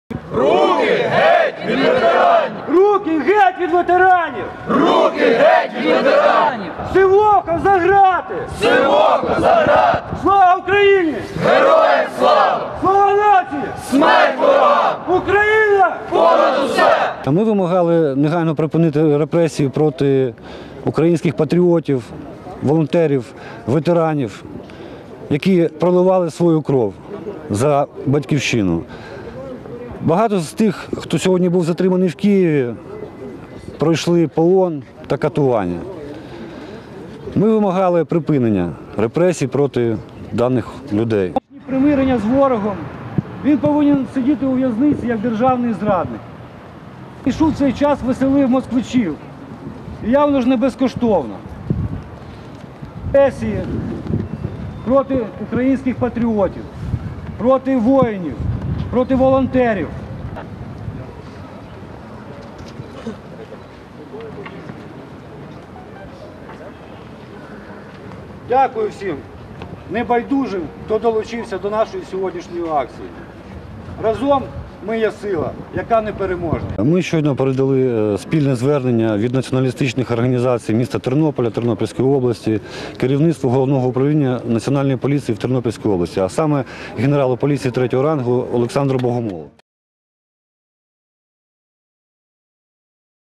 Біля управління Нацполіції в Тернополі відбулася акція "Руки геть від ветеранів"
На акцію «Руки геть від ветеранів» до управління Нацполіції в Тернополі прийшли 25 представників націоналістичних організацій.

Про це повідомив виконувач обов’язків голови Тернопільського осередку Нацкорпусу Олександр Пастух.

Вони вимагали закрити кримінальну справу за статтею «Хуліганство» 10-ти членам Нацкорпусу, які вдень зірвали презентацію Сергія Сивоха в Києві